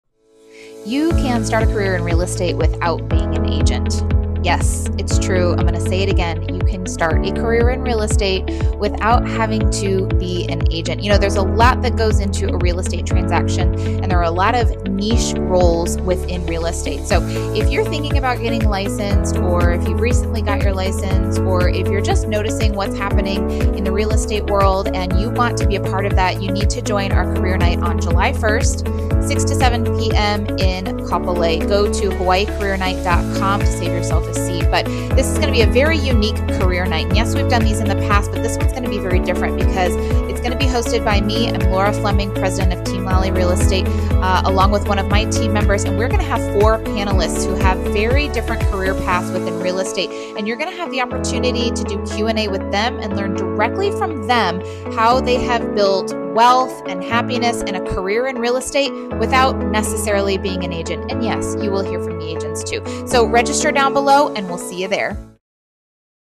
5 Days Away!!! Register Now At HawaiiCareerNight.com! | Team Lally
Our ALL NEW HAWAII CAREER NIGHT is just 5 days away!

Whether you want to learn about being an agent or one of the other career paths in real estate, we’ll have all the info you want on July 1st starting at 6 pm.

💻 HawaiiCareerNight.com to save yourself a seat!